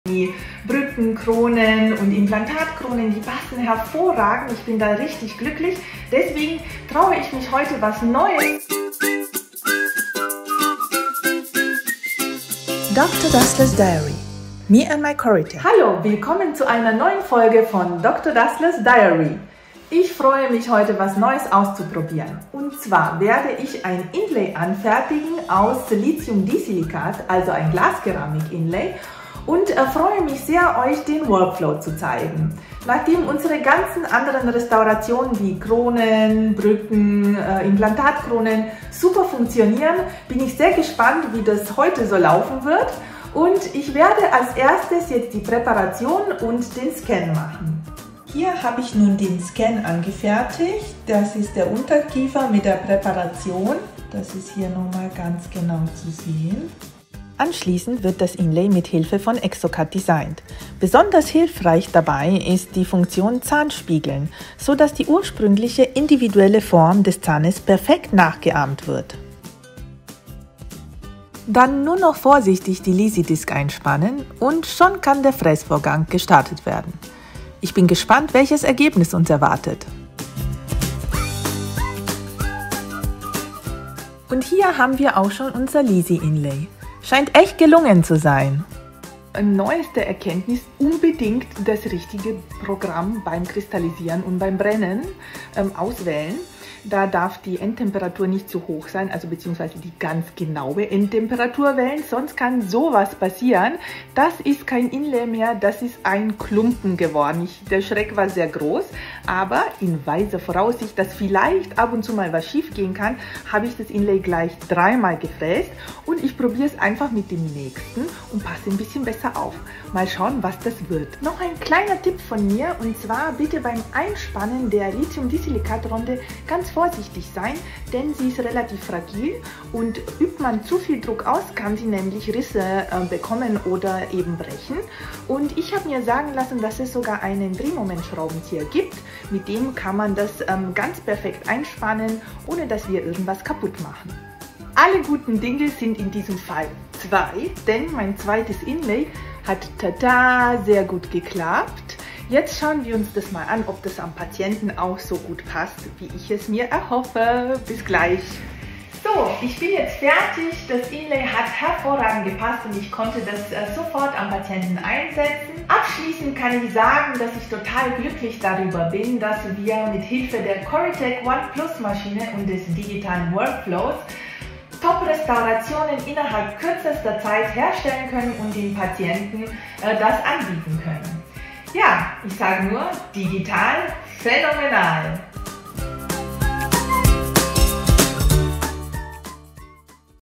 EPISODE 7 of Dr. Dassler's Diary: Ready for New Challenges
An inlay made from a glass ceramic blank 🤩
Dr. Dassler has gained a lot of confidence in the production of crowns, bridges, and implant crowns in recent months.
Now, she faces a new challenge as she processes a glass ceramic blank for the first time in dental prosthetics manufacturing.

She experiences an 'aha' moment and shares her latest learnings and insights with you once again
👉 Tune in to EPISODE 7 of Dr. Dassler's Diary: Ready for New Challenges.

🎬 By the way: Our dental series is also available on TikTok and YouTube.